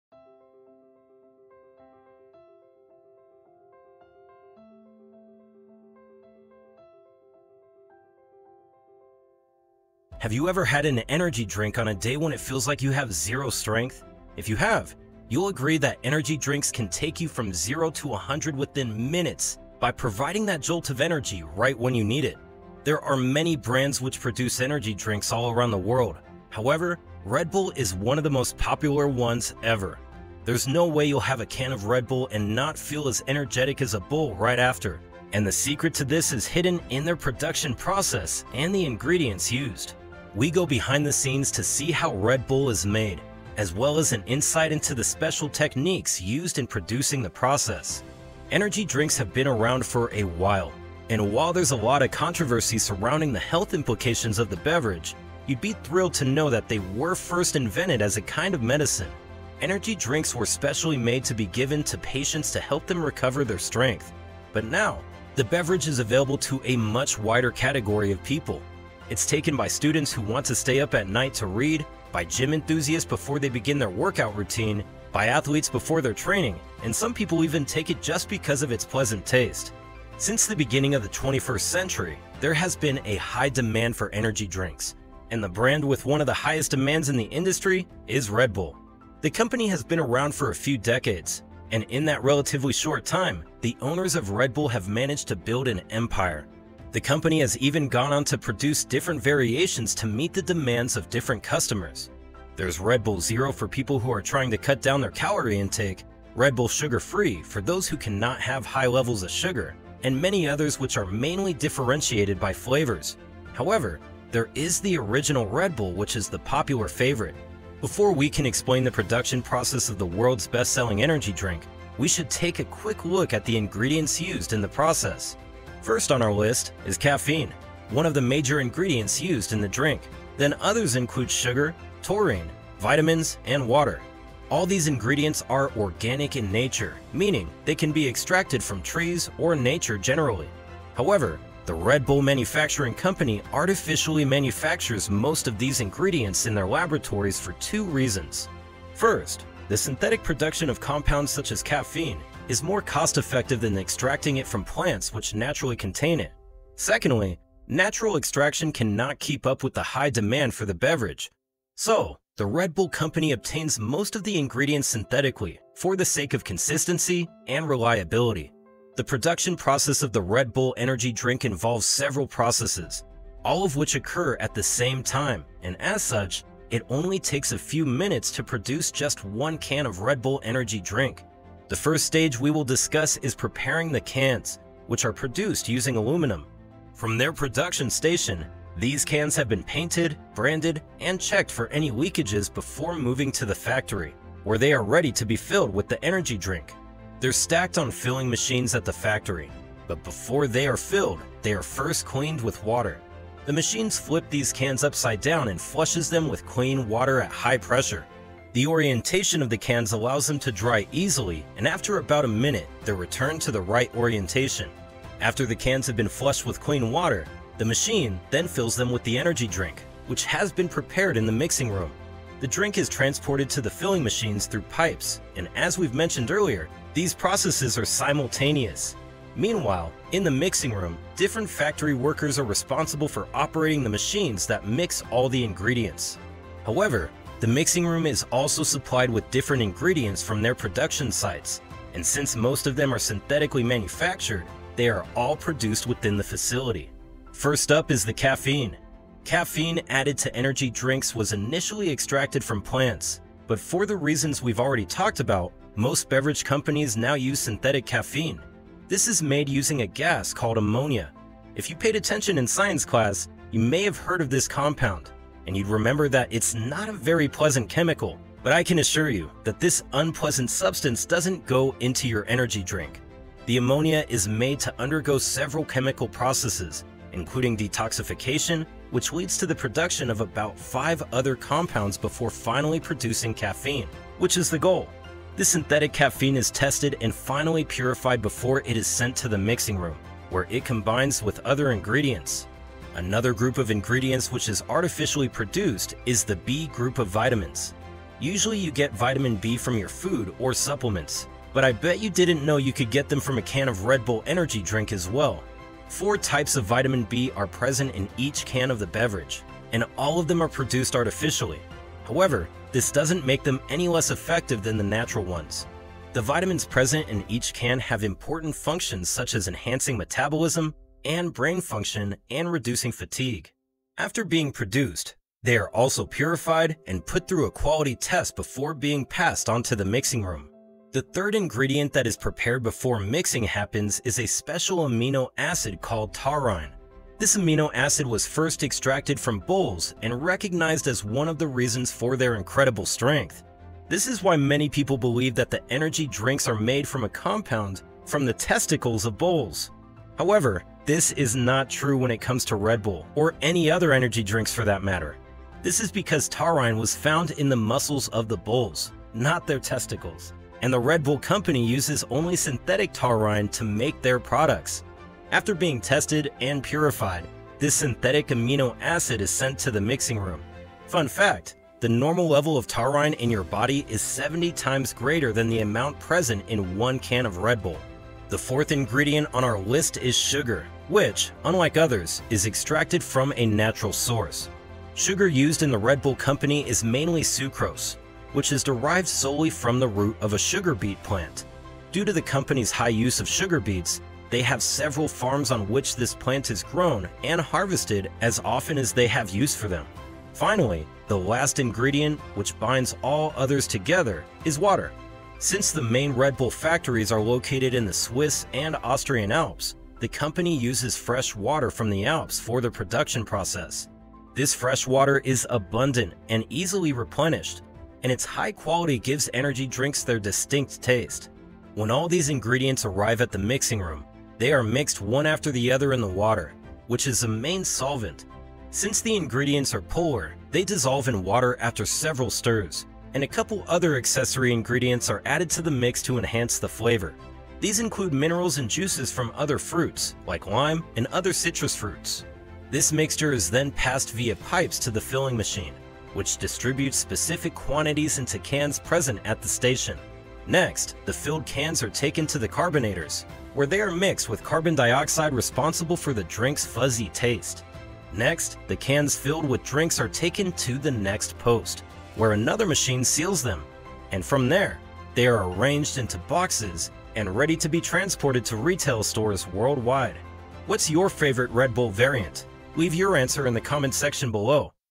Ever Wondered How Red Bull Is Made?! Join us on this FanTECHstic Factory Tour!
How Red Bull is made! Curious about how Red Bull is made? Get ready to discover how Red Bull is made!